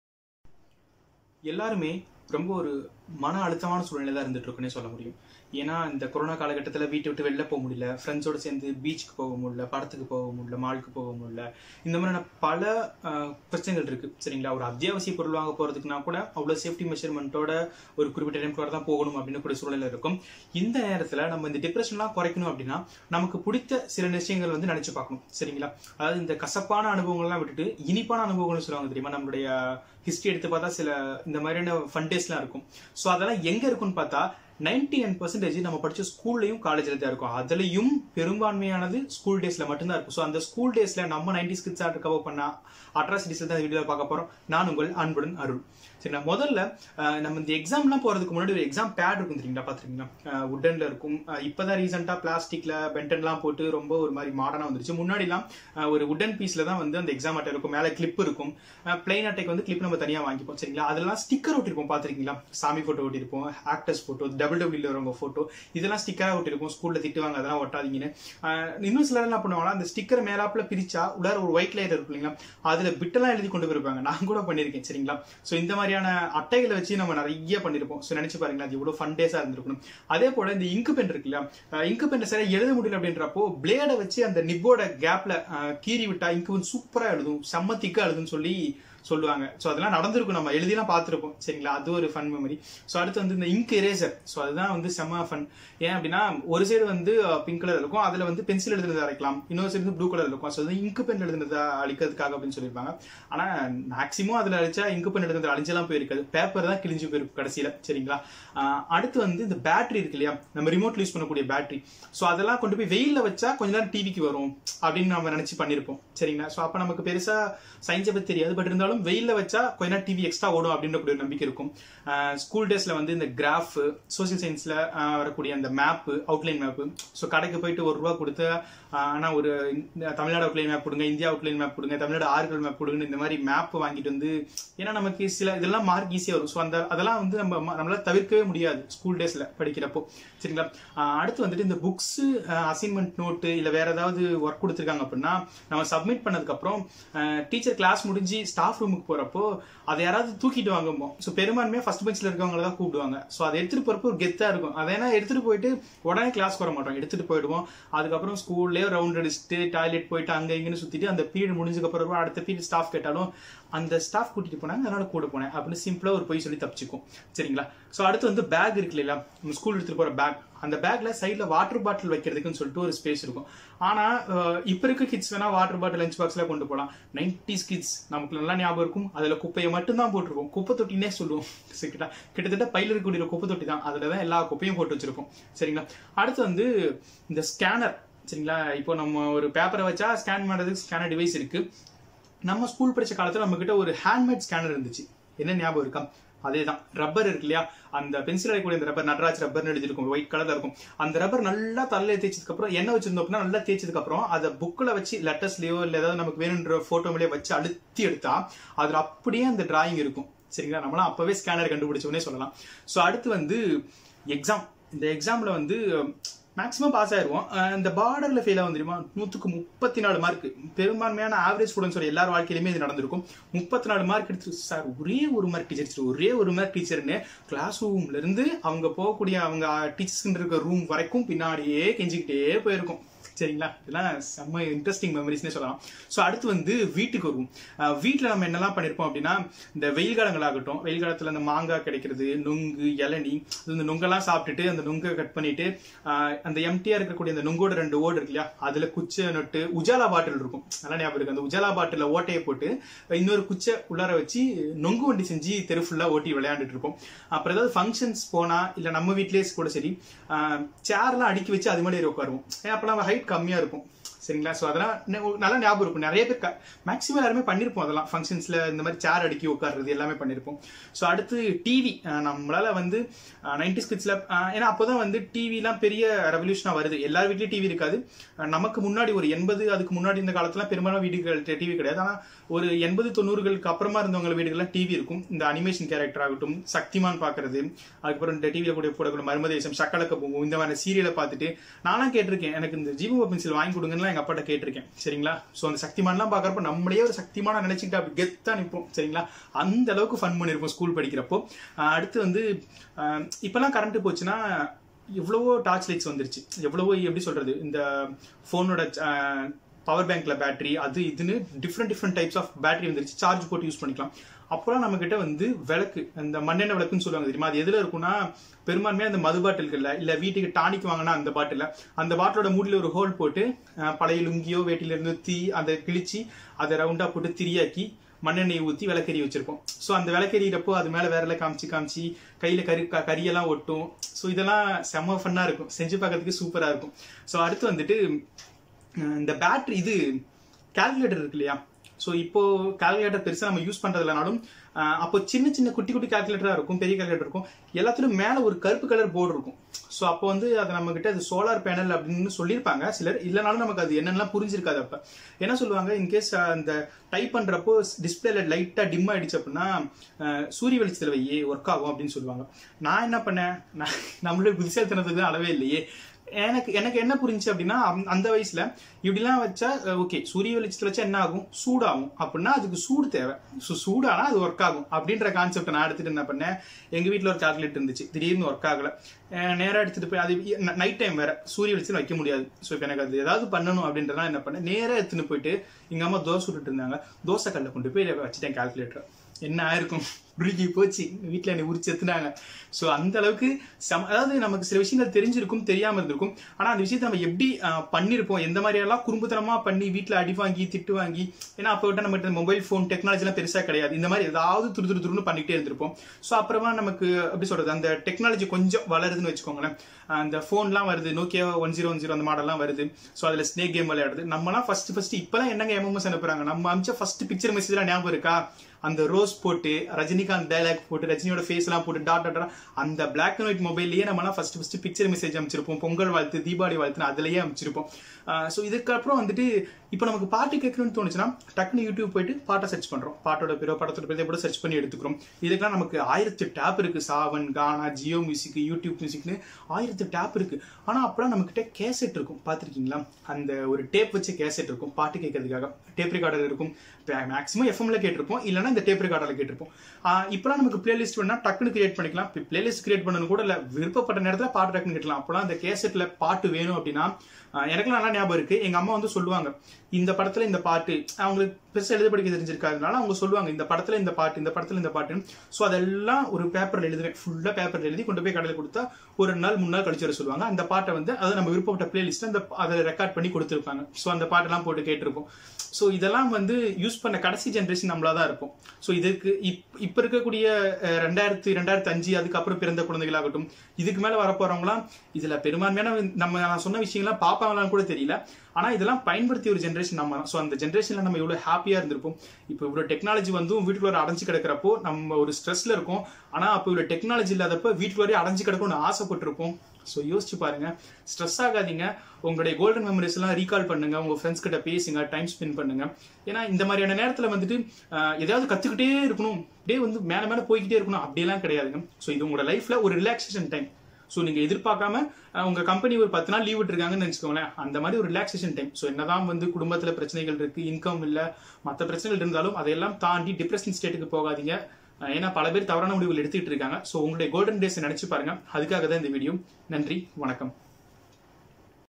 '90s Kids கட்டாயம் பார்க்க வேண்டிய Video | '90s Kids Memories | மீண்டும் பள்ளிக்கு போகலாம் | By Arul
மனஅழுத்தம் குறைய நம் வாழ்வில் நடந்த மகிழ்ச்சியான தருணங்களை நினைத்து பாருங்கள்.

20 வருடங்களுக்கு முன்பு 90s Kids எனப்படும் இளைஞர்கள் மறக்கமுடியாத சில நினைவுகளின் தொகுப்பு.
எனக்கு நியாபகம் இருந்த சில விஷயங்களை பகிர்ந்துள்ளேன். உங்களுக்கு நினைவுள்ளவற்றை என்னுடன் கம்மென்ட் இல் பகிருங்கள்.

Thanks for the Memes : 90s_Pasangadaw page